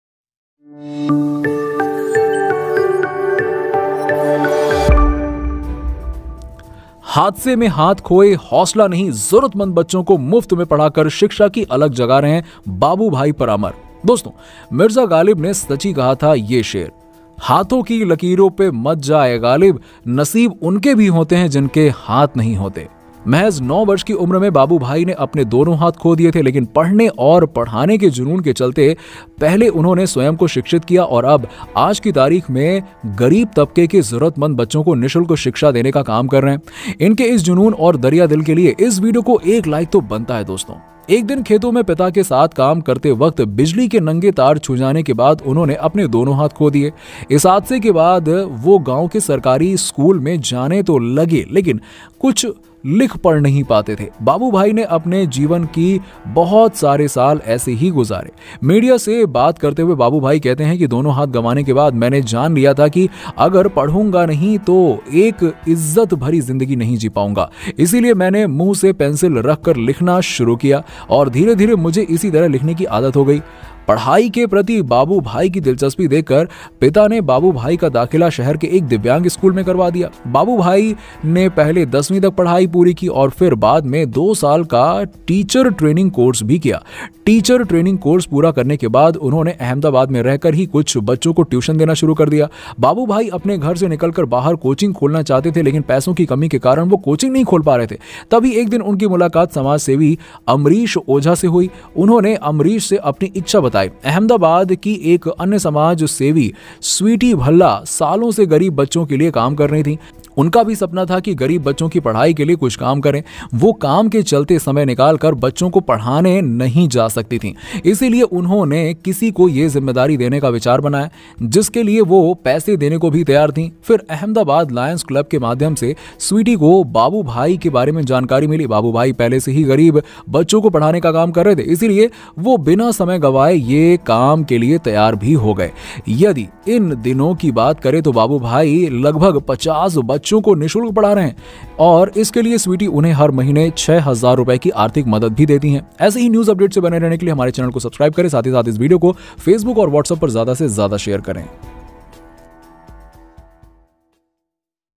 सब बच्चे इस शख्स को बोल रहे थे भगवान, जब वजह सामने आई तो होश उड़ गए सबके...
inspiration-babu-bhai-parmar-ahmedabad-teaching-poor-students.

Babu Bhai was born in a village situated on the edge of Ahmedabad city in the state of Gujarat. His father used to make a living by working in the fields, due to which Babu Bhai had seen poverty very closely since childhood. One day while working with his father in the fields, he lost both his hands after touching a bare electric wire.
After this accident, he had started going to the government school in the village but could not read and write anything. Babu Bhai spent many years of his life like this. Slowly time was passing and this thing was eating them inside. By now Babu Bhai had realized that if he kept sitting like this, then life would become very difficult.

Talking to the media, Babu Bhai says, "After losing both hands, I knew that if I do not study, I will not be able to lead a respectable life. So I started writing with a pencil in my mouth and slowly I started writing like this. Got used to.